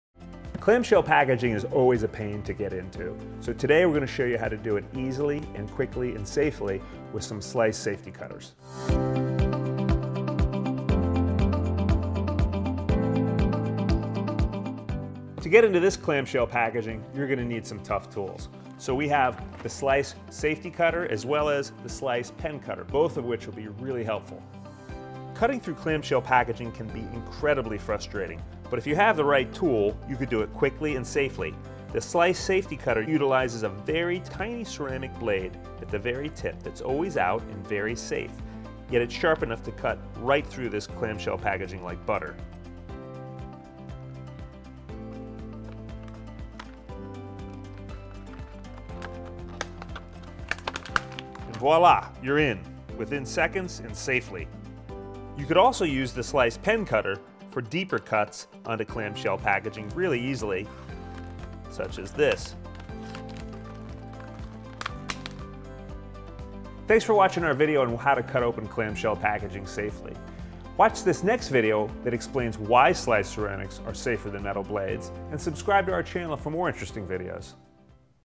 Slice Safety Cutter Opens Clamshell Packaging
Slice founder TJ Scimone explains how to open clamshell packaging—the clear, formed plastic that's notoriously hard to get into—safely and easily. Scimone demonstrates the Slice Safety Cutter and Pen Cutter.